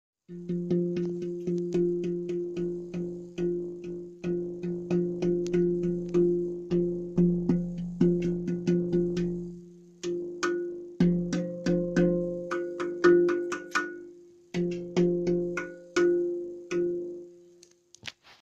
Atom Handpan tuning
Sorry for quality, video from phone
Atom handpan
Maker: Luchentsov Yevgeny, Ukraine